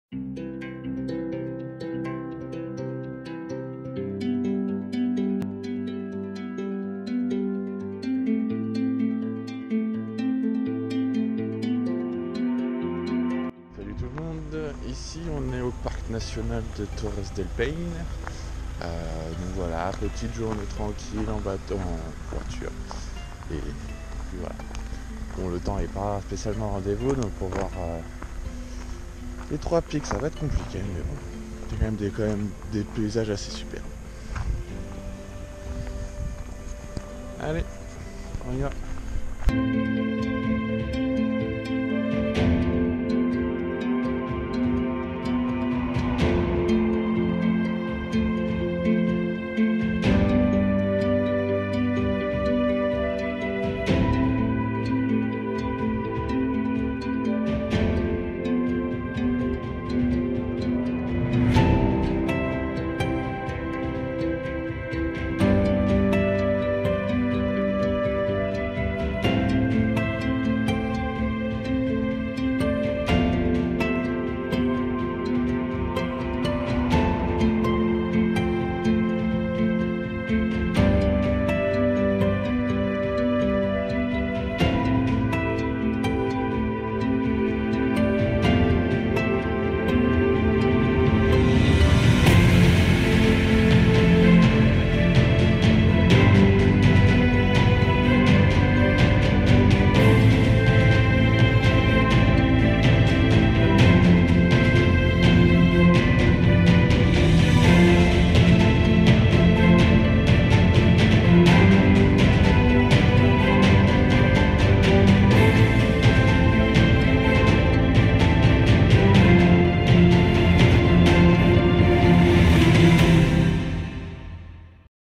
J'ai réussi à apercevoir les trois pics à Torres del Paine !! Chili - Avril 2019
Première étape au Chili au parc national de Torres del Paine...

First step in Chile at the national park of Torres del Paine...

Erste Etape in Chile in den National Park Torres del Paine...